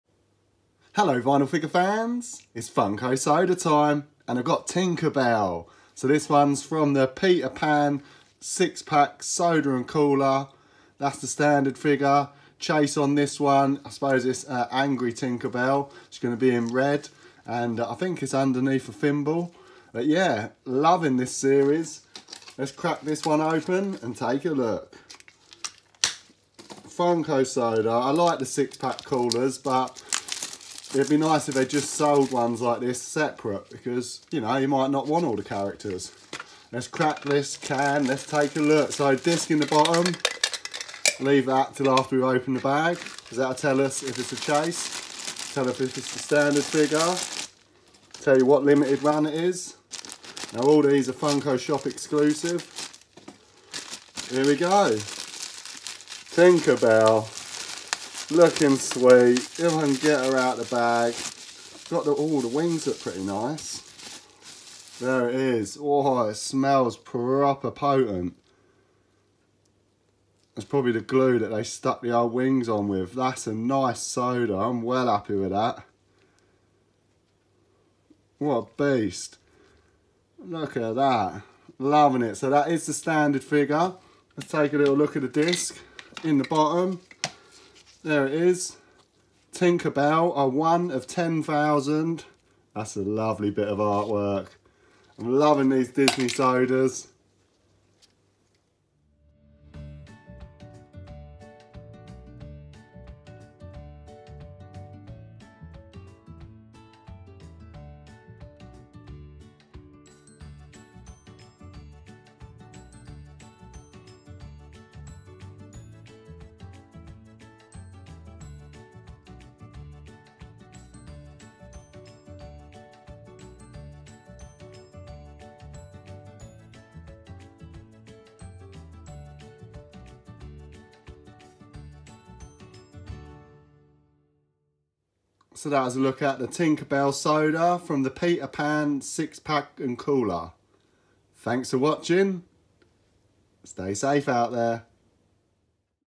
Tinkerbell Funko Soda Figure
Can opening and figure review of Tinkerbell from Disneys Peter Pan Funko Soda 6 pack and cooler Funko Shop Exclusive.